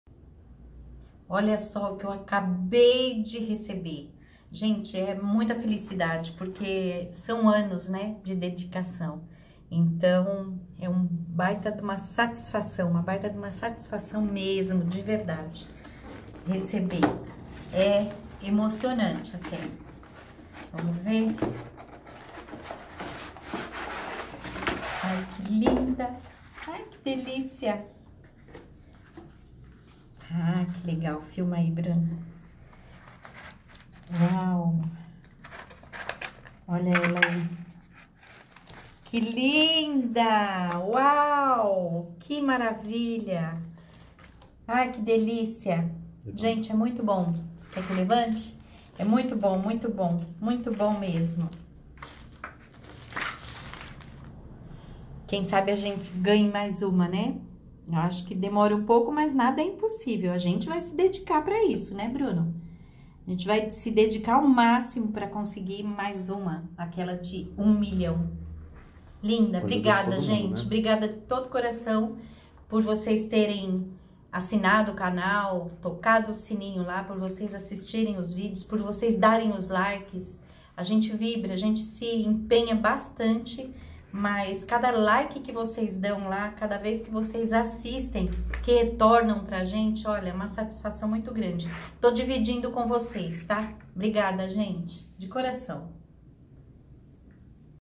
OBRIGADA! A placa pelos mais de 100.000 inscritos! VEM NOVIDADE POR AÍ!
Um conquista pela qual devo a vocês: a placa de 100.000 inscritos no meu canal no YouTube. E já somos mais de 120.000 agora.

A todos que se inscreveram: obrigada! Vocês fazem parte dessa história. É por vocês, e pra vocês. Sempre.

Temos sempre pelo menos um vídeo novo por semana, uma aula que pensamos, executamos e gravamos pra vocês. E sempre de graça.

Na semana que vem farei uma série de programas ao vivo aqui no YouTube (e também no meu Instagram @elianazerbinatti).

Mais de 70% das pessoas votaram nos meus stories e na minha página no Facebook e definiram que o melhor período para essas lives é o noturno.

E o horário, qual o melhor? Vote nos meus stories ou lá no Facebook, ou comente aqui. Faço às 19h ou às 22h?